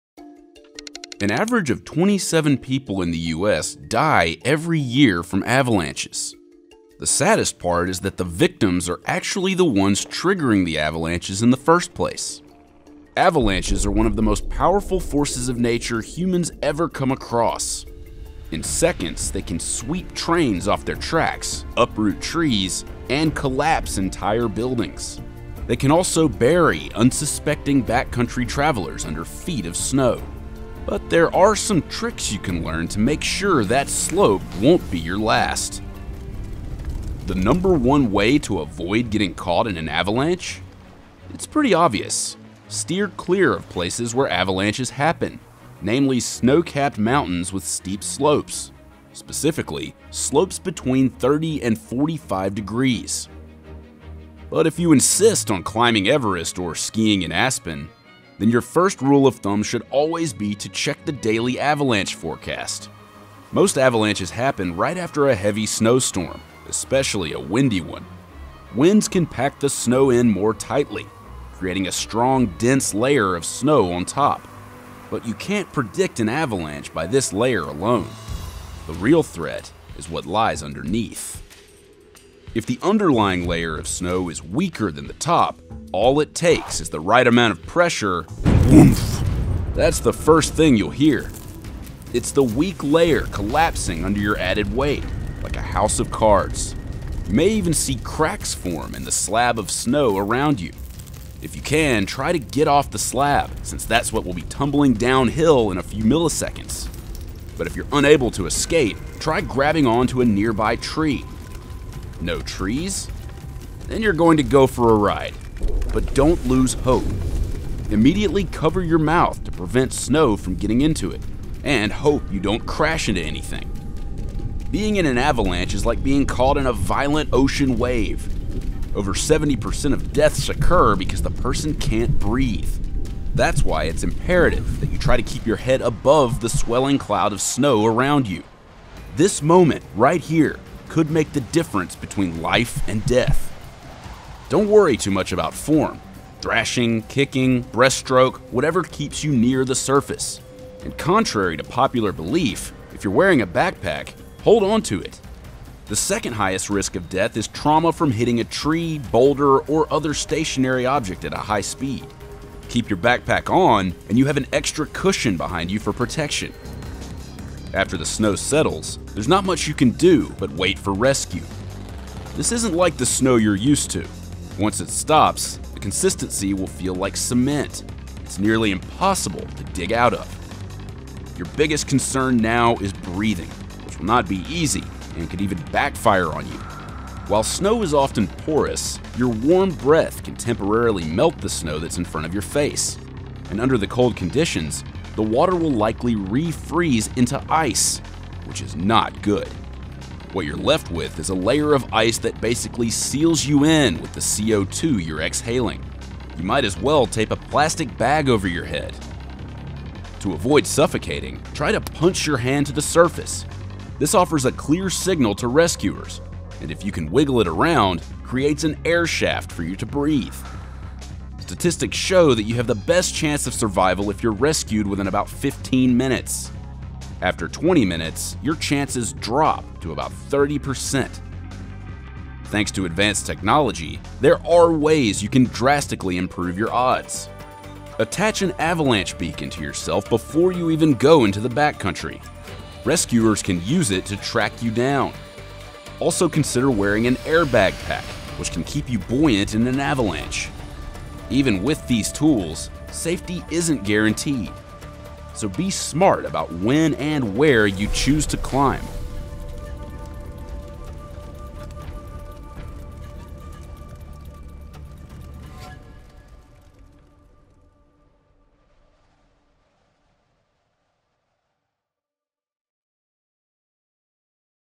How To Survive An Avalanche
Going to the snowy mountains is not all fun. Amidst it all lies avalanches so strong they can uproot anything that blocks their path. But here are some ways you can survive the deadly grip of an avalanche.

Following is the transcript of the video:

An average of 27 people in the US die every year from avalanches. The saddest part is that the victims are actually the ones triggering the avalanches in the first place.

Avalanches are one of the most powerful forces of nature humans ever come across. In seconds, they can sweep trains off their tracks, uproot trees, and collapse entire buildings. They can also bury unsuspecting backcountry travelers under feet of snow. But there are some tricks you can learn to make sure that slope won’t be your last.

The number one way to avoid getting caught in an avalanche? It’s pretty obvious steer clear of places where avalanches happen, namely snow-capped mountains with steep slopes. Specifically, slopes between 30-45 degrees.

But if you insist on climbing Everest or skiing in Aspen, then your first rule of thumb should always be to check the daily avalanche forecast. Most avalanches happen right after a heavy snowstorm especially a windy one. Winds can pack the snow in more tightly, creating a strong, dense layer of snow on top.

But you can’t predict an avalanche by this layer, alone. The real threat is what lies underneath. If the underlying layer of snow is weaker than the top, all it takes is the right amount of pressure and WHUMPF!

That’s the first thing you’ll hear. It’s the weak layer collapsing under your added weight, like a house of cards. You may even see cracks form in the slab of snow around you. If you can, try to get off the slab, since that’s what will be tumbling downhill in a few milliseconds.

But if you’re unable to escape, try grabbing on to a nearby tree. No trees?

Then you’re going to go for a ride. But don’t lose hope! Immediately cover your mouth to prevent snow from getting into it and hope you don’t crash into anything.

Being in avalanche is like being caught in a violent ocean wave. Over 70% of deaths occur because the person can’t breathe. That’s why it’s imperative that you try to keep your head above the swelling cloud of snow around you. This moment, right here, could make the difference between life and death.

Don’t worry too much about form. Thrashing, kicking, breaststroke whatever keeps you near the surface. And contrary to popular belief, if you’re wearing a backpack, hold on to it!

The second-highest risk of death is trauma from hitting a tree, boulder, or another stationary object at a high speed. Keep your backpack on, and you have an extra cushion behind you for protection.

After the snow settles, there’s not much you can do but wait for rescue.

This isn’t like the snow you’re used to once it stops, the consistency will feel like cement. It’s nearly impossible to dig out of.

Your biggest concern now is breathing which will not be easy, and could even backfire on you. While snow is often porous, your warm breath can temporarily melt the snow that’s in front of your face. And under the cold conditions, that water will likely re-freeze into ice, which is not good.

What you’re left with is a layer of ice that basically seals you in with the CO2 you’re exhaling. You might as well tape a plastic bag over your head.

To avoid suffocating, try to punch your hand to the surface. This offers a clear signal to rescuers and if you can wiggle it around creates an air shaft for you to breathe.

Statistics show that you have the best chance of survival if you’re rescued within about 15 minutes. After 20 minutes, your chances drop to about 30%.

Thanks to advanced technology, there are ways you can drastically improve your odds. Attach an avalanche beacon to yourself before you even go into the backcountry. Rescuers can use it to track you down. Also consider wearing an airbag pack, which can keep you buoyant in an avalanche.

Even with these tools, safety isn’t guaranteed. So, be smart about when and where you choose to climb.